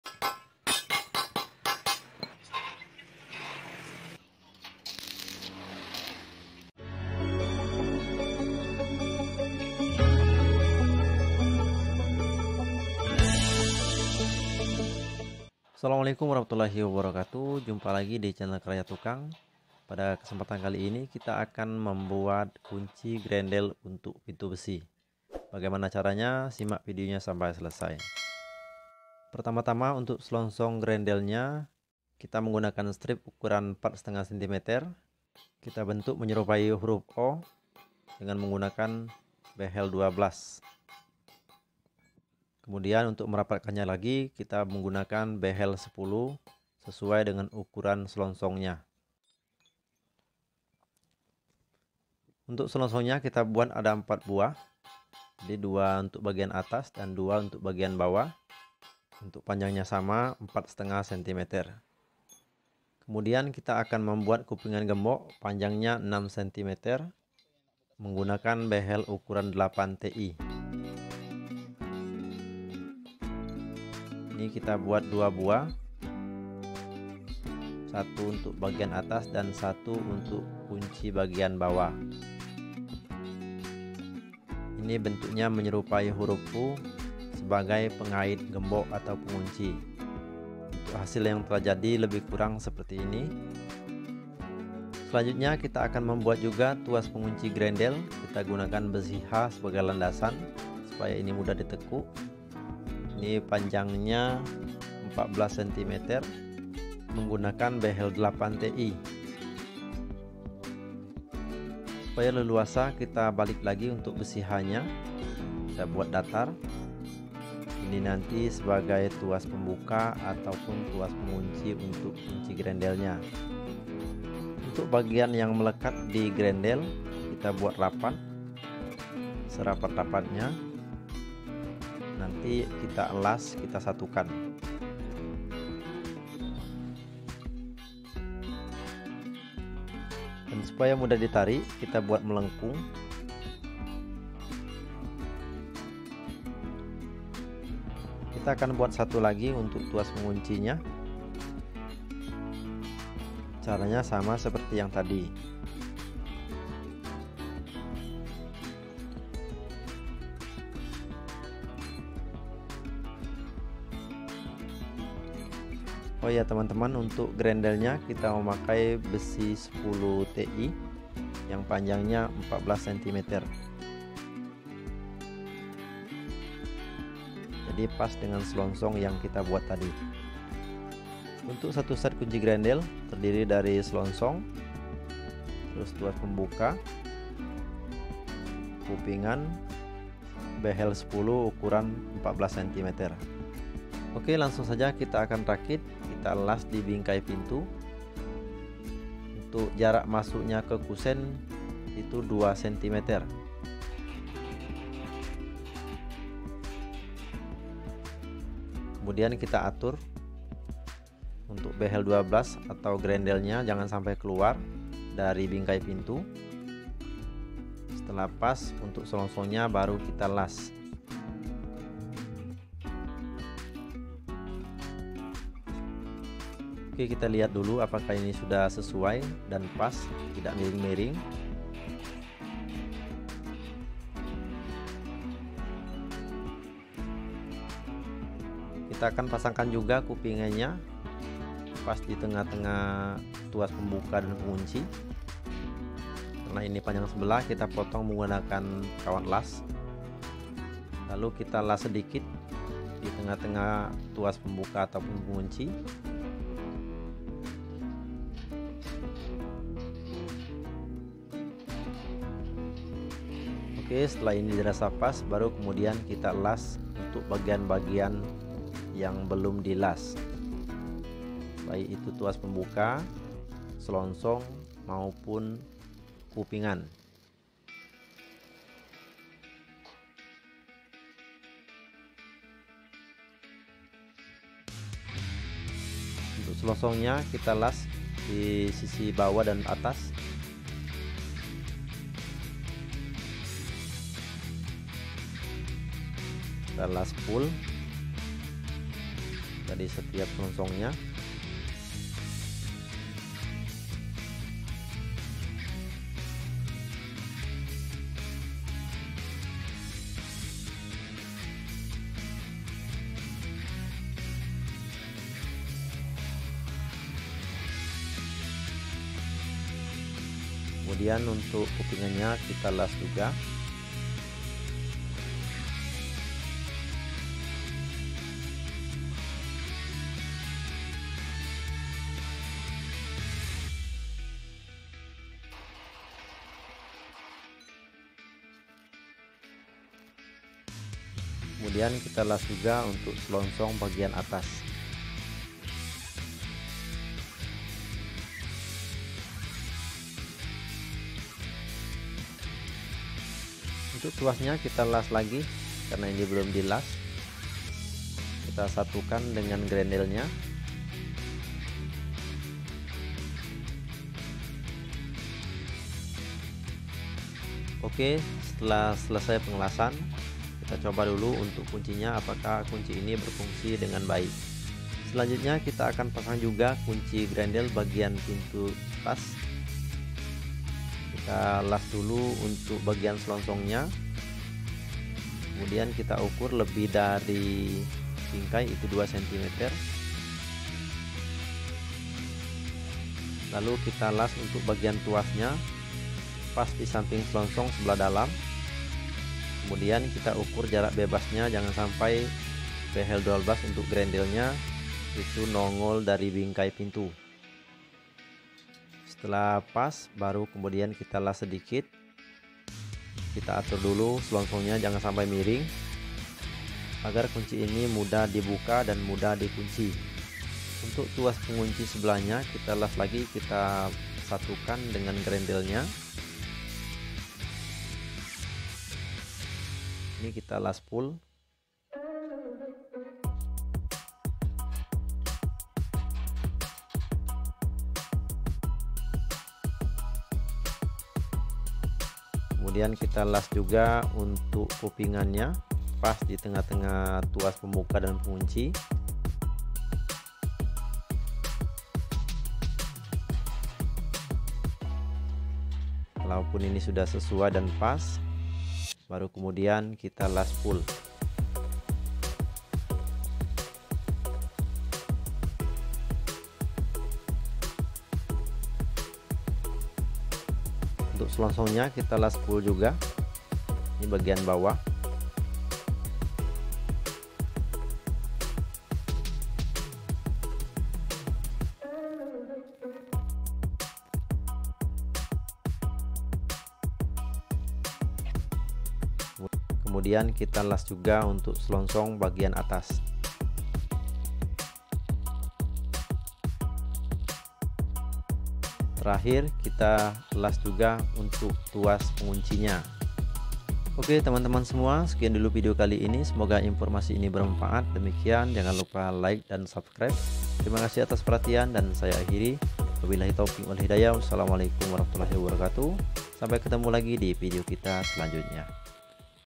Cara Membuat Dan Memasang Kunci Grendel Pada Pintu Besi
Halo sobat semua mudah mudahan sehat wal'afiat dan dilancarkan rizkinya oleh ALLAH SWT amiin.

Pada vedeo kali ini kita akan berbagi tips cara membuat dan memasang kunci grendel pada pintu besi.

Ukurannya sbb :
Strip selongsong panjang 4,5 cm
Tuas pengunci panjang 14 cm dari behel 8 ti
Kupingan panjang 6 cm dari behel 8 ti
Grendel panjang 14 cm dari behel 10 ti

Untuk cara membuat dan memasang nya yuk kita saksikan saja vedeo nya sampai selesai.